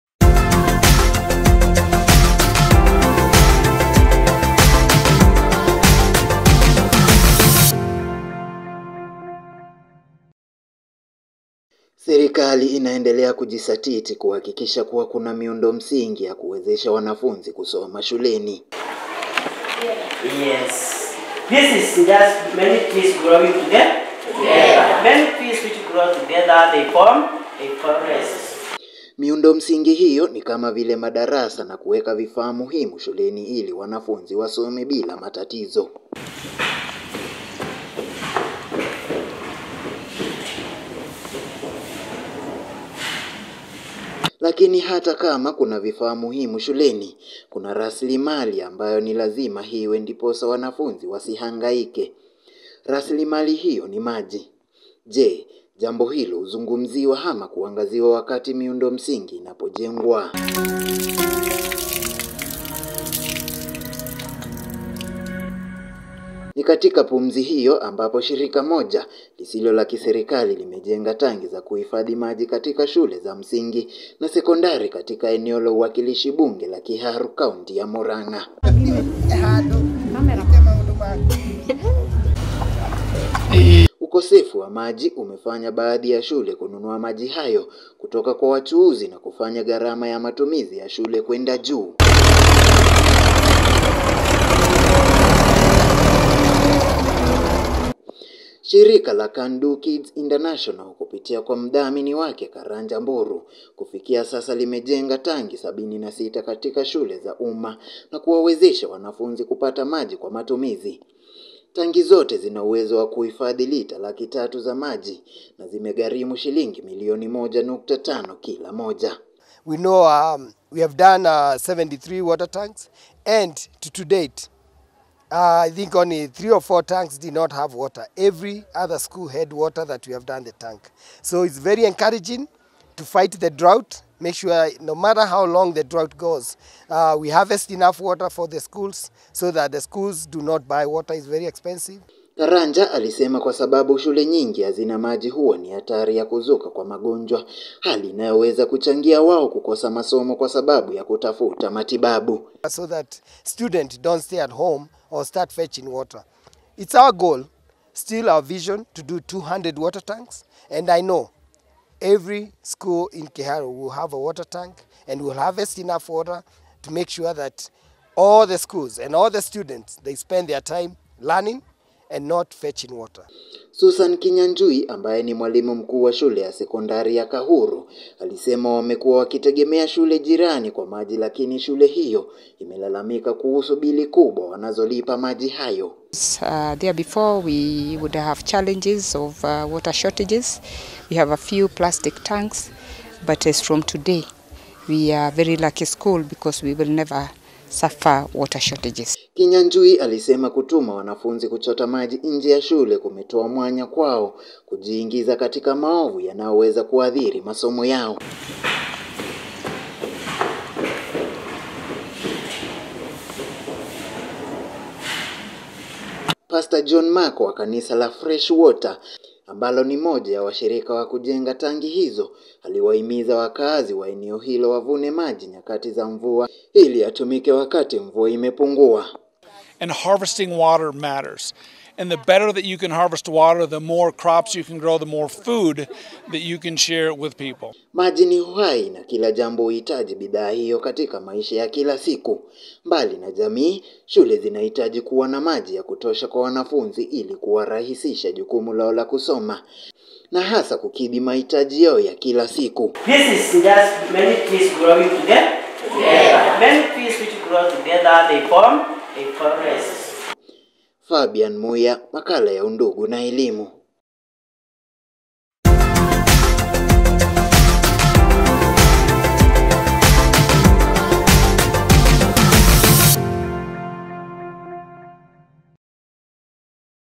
HITAJI LA MAJI SHULENI
Serikali yaendela kujenga miundo msingi shuleni ili kukidhi mahitaji mengi